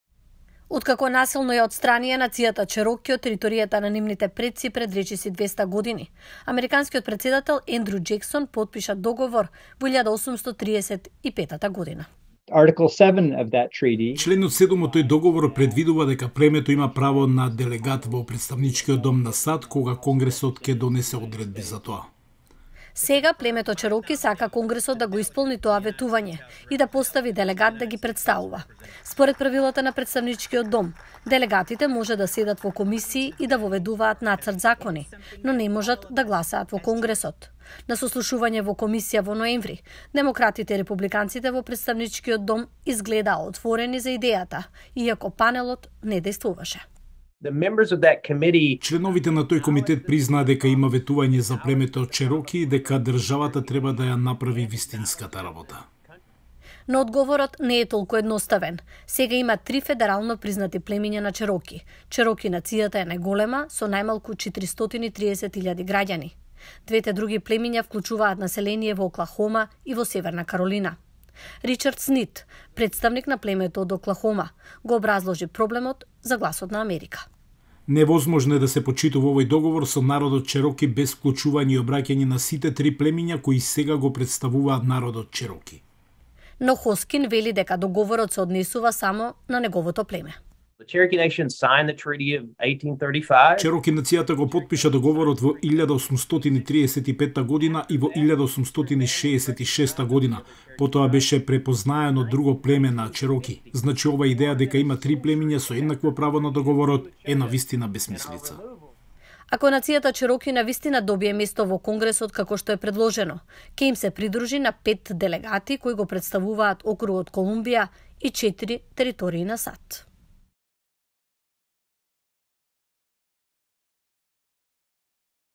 Домородниот американски народ Чероки бара место во Конгресот на САД
Пред скоро 200 години, американската влада му вети на домородниот народ Чероки место во Конгресот во замена за нивната територија. Досега нема испорачано. Чероки сега го повикува Конгресот да го исполни тоа ветување, но има несогласување околу тоа кое од трите племиња на Чероки да го добие местото.